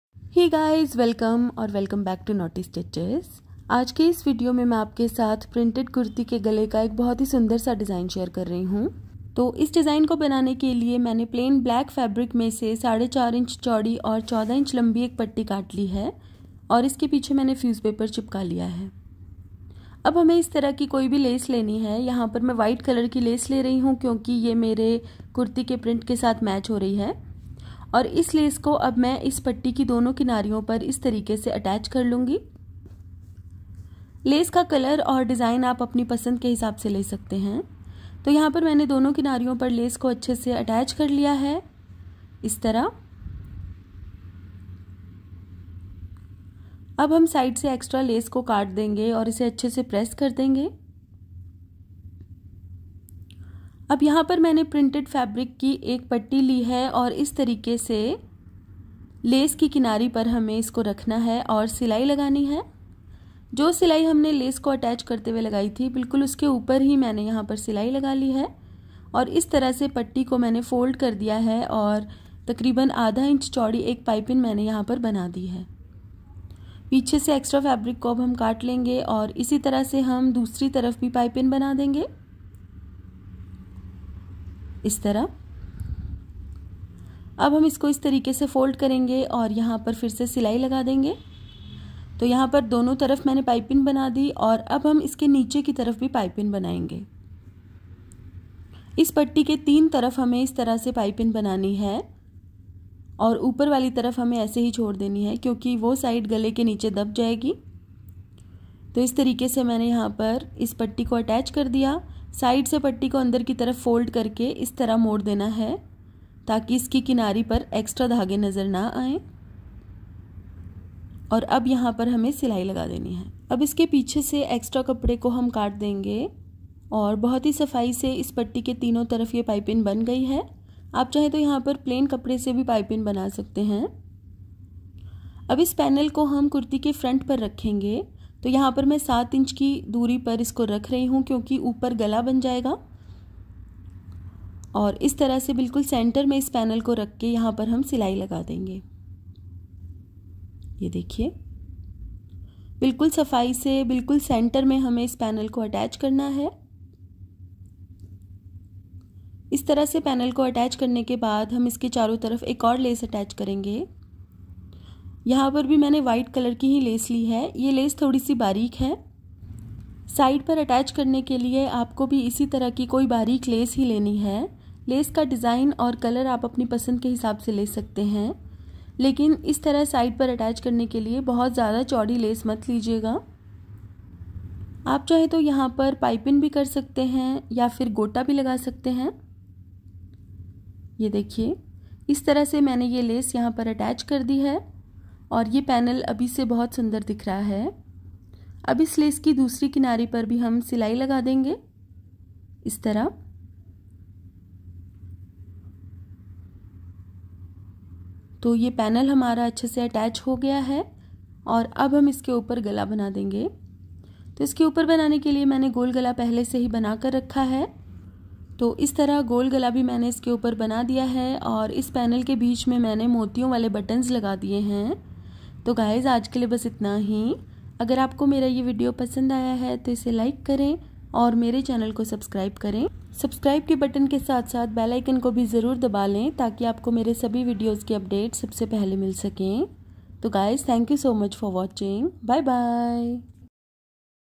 Printed Fabric और Lace से Patchwork कर के बनाएं Printed Kurti के गले का Simple और Beautiful Design
Check out other videos as well:-